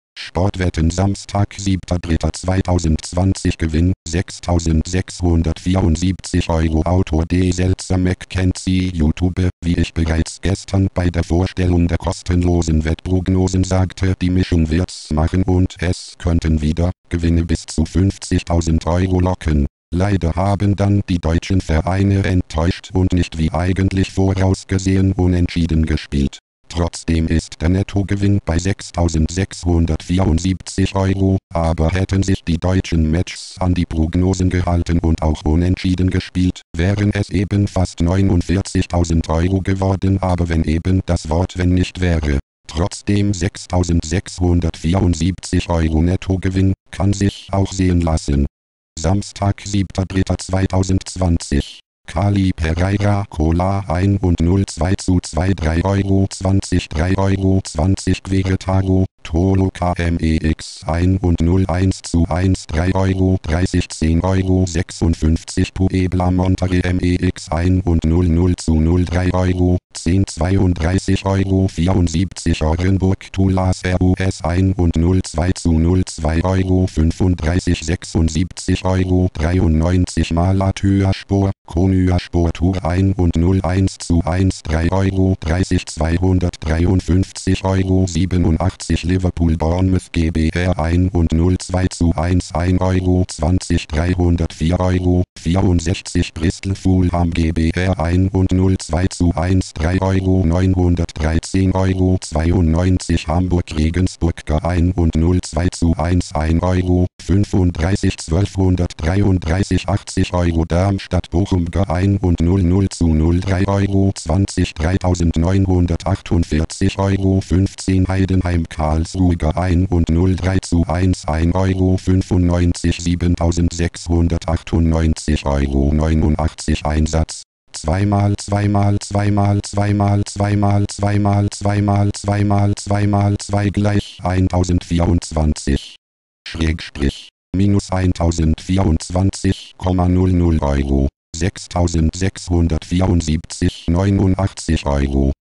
Sportwetten Samstag 7.3.2020 Gewinn: € 6.674.—
Author D. Selzer-McKenzie
Wie ich bereits gestern bei der Vorstellung der kostenlosen Wettprognosen sagte, die Mischung wird’s machen und es könnten wieder Gewinne bis zu 50000 Euro locken. Leider haben dann die deutschen vereine enttäuscht und nicht wie eigentlich vorausgesehen, unentschieden gespielt. Trotzdem ist der Nettogewinn bei € 6674.--, aber hätten sich die deutschen Matchs an die Prognosen gehalten und auch unentschieden gespielt, wären es eben fast € 49000.—geworden, aber wenn eben das Wort wenn nicht wäre. Trotzdem, € 6674.—Nettogewinn kann sich auch sehen lassen.

Samstag 7.3.2020
Cali Pereira COLA 1 und 0 2:2 3,20 € 3,20 €
Queretaro Toluca MEX 1 und 0 1:1 3,30 € 10,56 €
Puebla Monterrey MEX 1 und 0 0:0 3,10 € 32,74 €
Orenburg Tulas RUS 1 und 0 2:0 2,35 € 76,93 €
Malatyaspor Konyaspor TUR 1 und 0 1:1 3,30 € 253,87 €
Liverpool Bournemouth GBR 1 und 0 2:1 1,20 € 304,64 €
Bristol Fulham GBR 1 und 0 2:1 3,00 € 913,92 €
Hamburg Regensburg GER 1 und 0 2:1 1,35 € 1.233,80 €
Darmstadt Bochum GER 1 und 0 0:0 3,20 € 3.948,15 €
Heidenheim Karlsruhe GER 1 und 0 3:1 1,95 € 7.698,89 €
Einsatz: 2x2x2x2x2x2x2x2x2x2 = 1024   ./. -1.024,00 €
      6.674,89 €